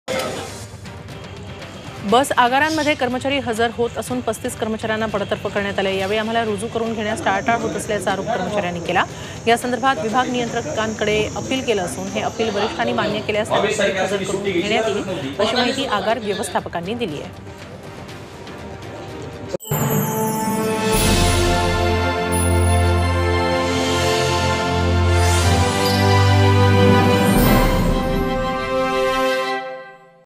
ST Workers | रुजू करुन घेण्यास टाळाटाळ, एसटी कर्मचाऱ्यांचा आरोप | Zee24Taas
5 Minitat 25 Batmya । 5 मिनिटांत 25 बातम्या